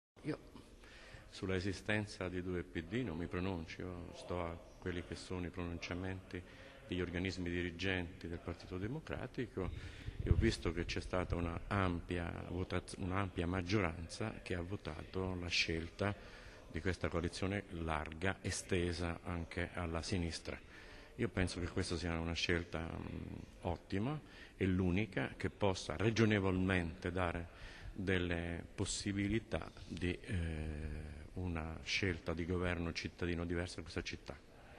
170108 (VI) Erasmo Venosi sul PD
La Sinistra Arcobaleno si presenta: a 6 mesi dall'appuntamento con le Elezioni Amministrative nel capoluogo, i rappresentanti di PdCI, Rifondazione Comunista, Sinistra Democratica e Verdi hanno illustrato alla stampa locale il documento su cui si baseranno le alleanze e la stesura del programma definitivo e, forse, unitario con le altre forze del centro sinistra.  Nella corsa a Palazzo Trissino, il dialogo con il PD vicentino è già aperto da settimane; pesano, pero', le polemiche del Commissario Straordinario al Dal Molin, Paolo Costa.